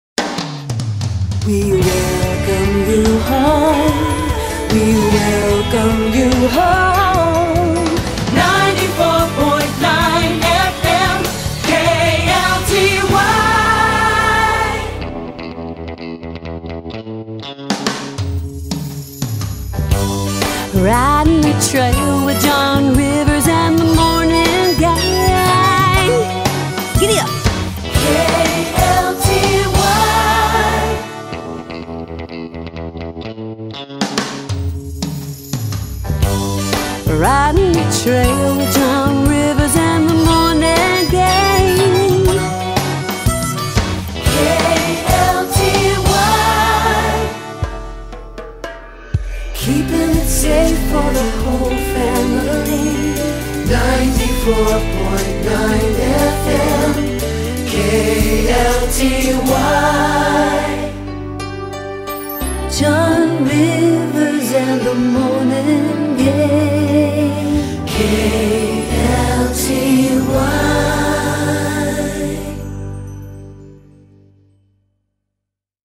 LISA BEVILL - “KLTY - 94.9 FM Christian Radio Station” Radio Promo's
LISA BEVILL - “KLTY - 94.9 FM Christian Radio Station”
Lisa Bevill Official YouTube Channel
Created by:  Chris Harris & Jon Rivers
KLTY-FM and Salem Interactive Media, Irving, TX

Jon Rivers is one my most favorite Christian radio hosts.. This is a medley of some of the radio promo’s I did for him.  I threw in a Reba McEntire type radio promo for him to use.. we always like to goof around in the studio.. just a sample in the middle… “giddy up”..